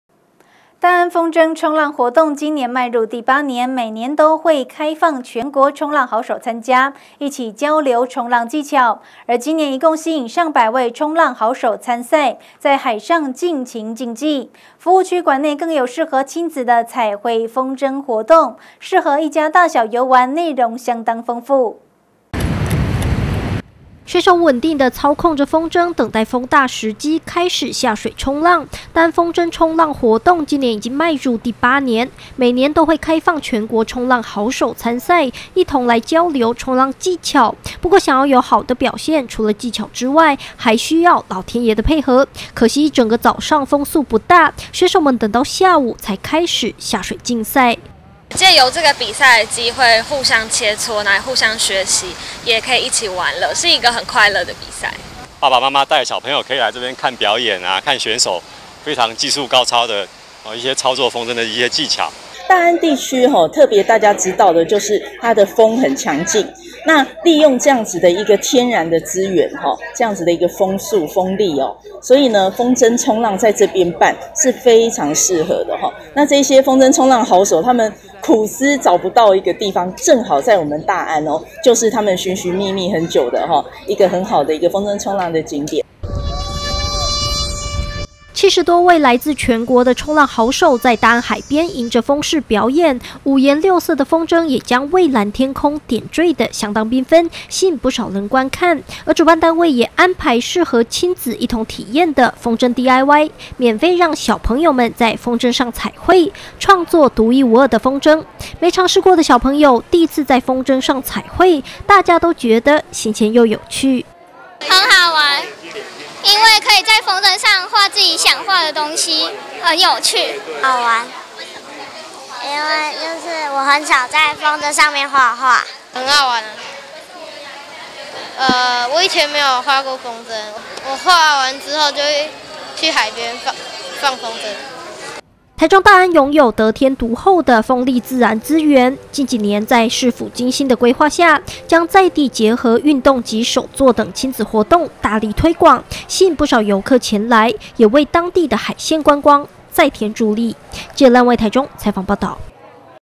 大台中新聞-風箏衝浪競賽 七十多位好手迎風炫技
大安風箏衝浪活動，今年已經邁入第八年，每年都會開放全國衝浪好手參賽，一同交流衝浪技巧。而今年一共吸引百多位的好手參賽，在海上盡情競技；服務區館內更有適合親子的彩繪風箏活動，適合一家大小遊玩，內容相當豐富。